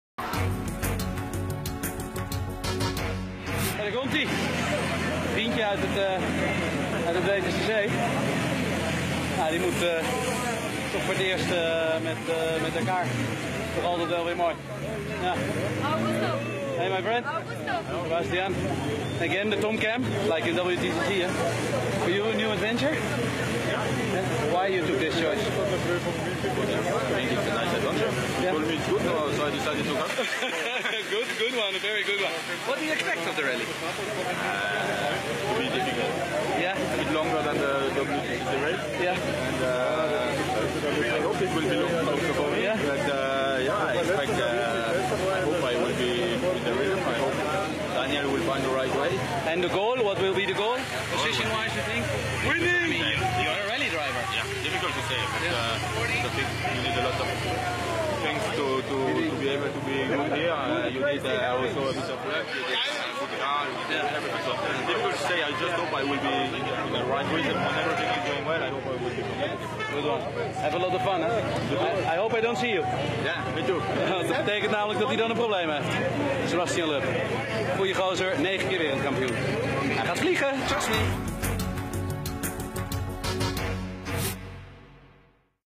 Tom Coronel Met Loeb at the admin checks! | Facebook
Met my FIA WTCC friend Sébastien Loeb at the administration checks 😄😜 Good to see you at the Dakar Rally 👊🏻Good luck my hero! Dakar2016 🇦🇷 RTL GP